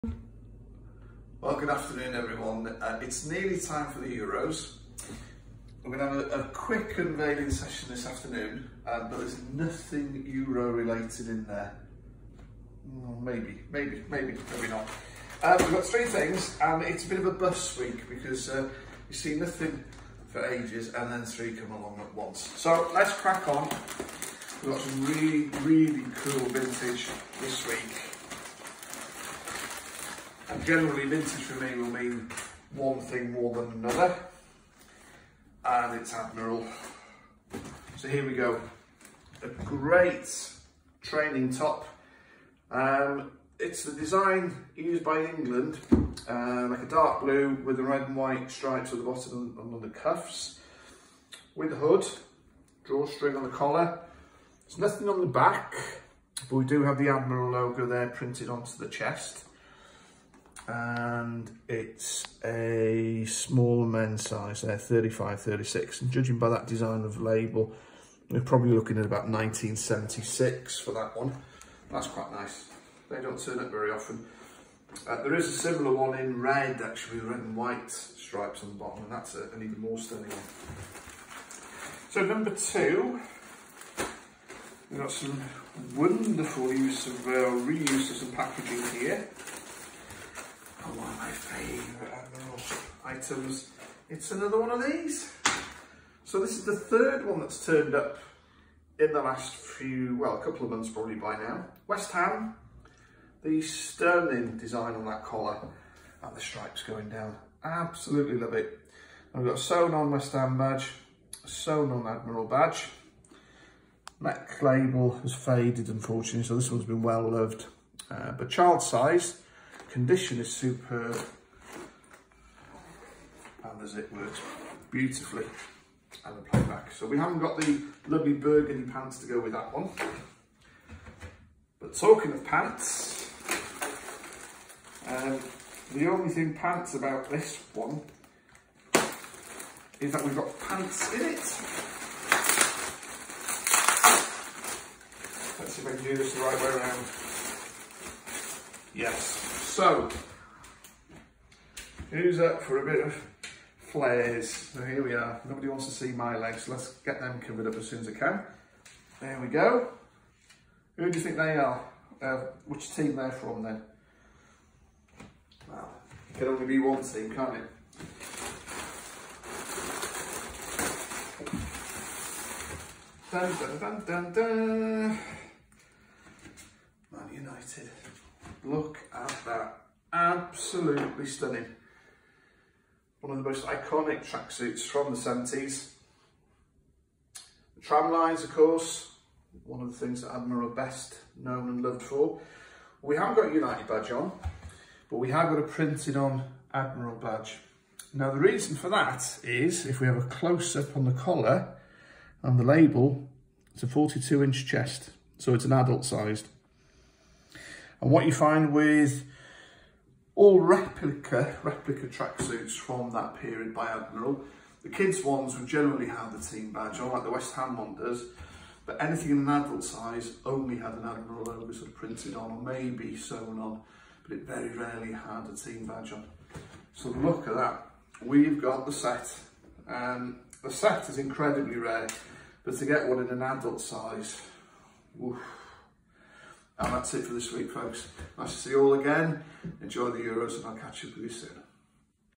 Original 1970s vintage Admiral tracksuit / tracker / track top unveiling
More original 1970s vintage sportswear being unveiled.
All are made by Admiral Sports.